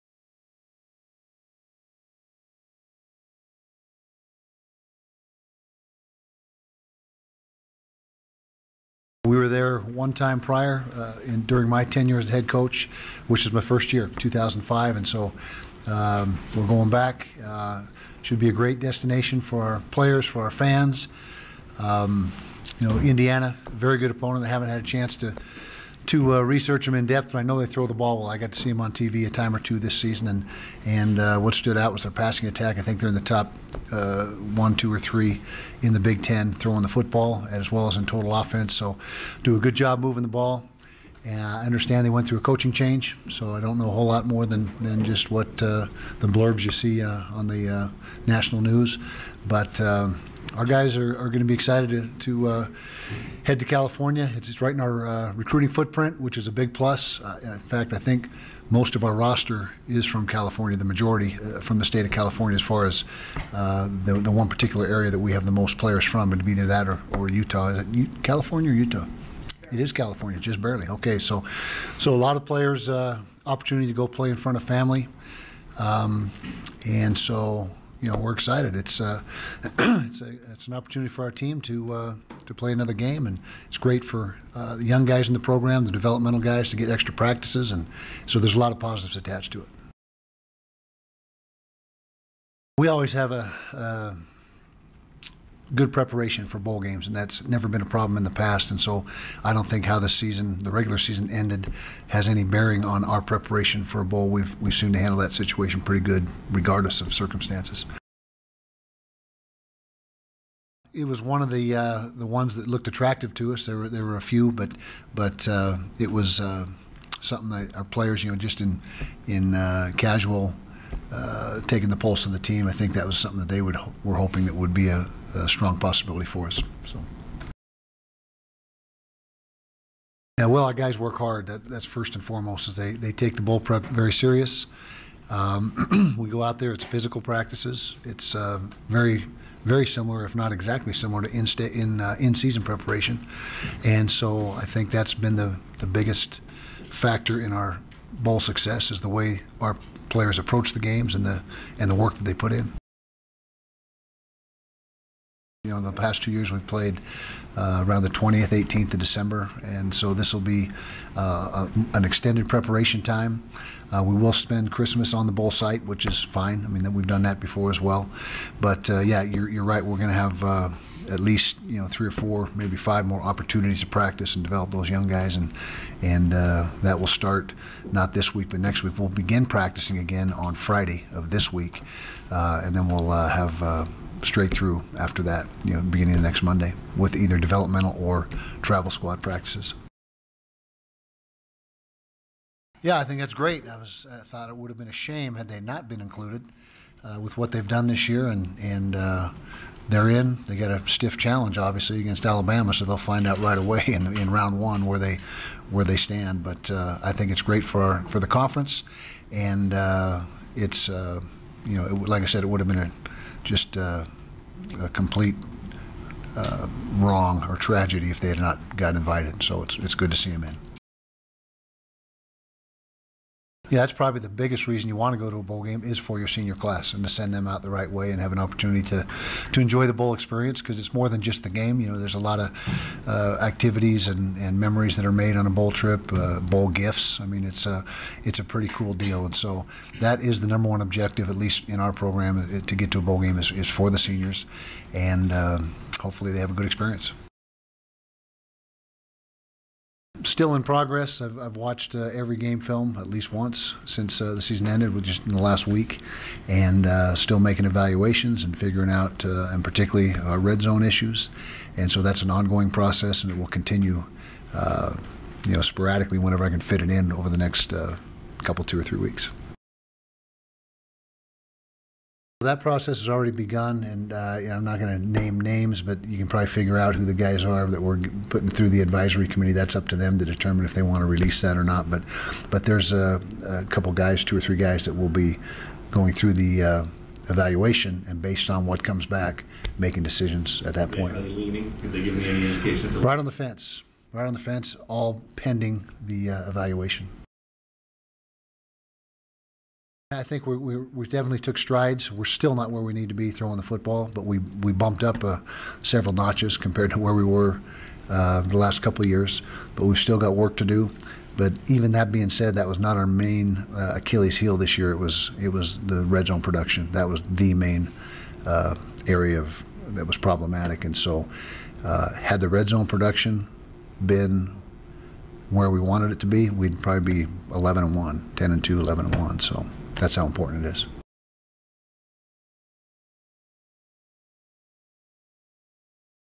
Utah coach Kyle Whittingham on the team's Foster Farms Bowl selection
Utah coach Kyle Whittingham discusses the Utes' Foster Farms Bowl selection on December 4th, 2016.

Video shot and edited by Brennan Smith.